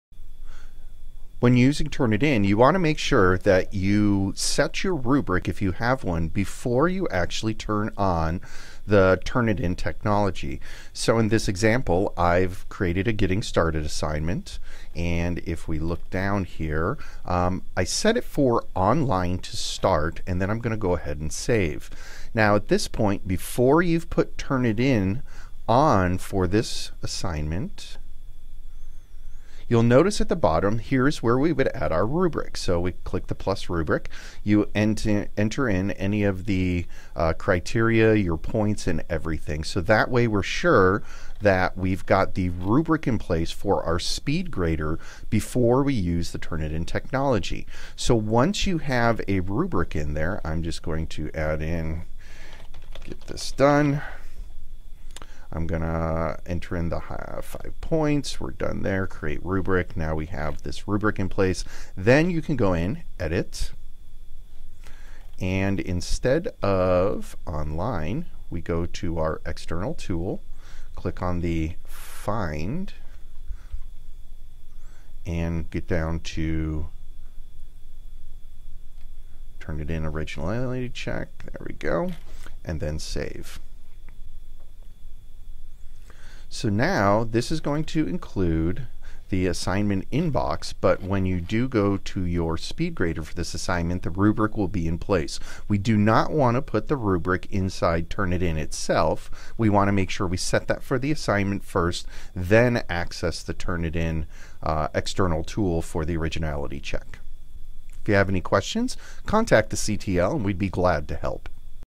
TurnitIn with Canvas Rubric - Setup
Make sure you create the assignement with the rubric in Canvas BEFORE setting the external tool to the TurnitIn Originality check. This allows you to get an originality score, but also use Speedgrader with the rubric to score the assignment.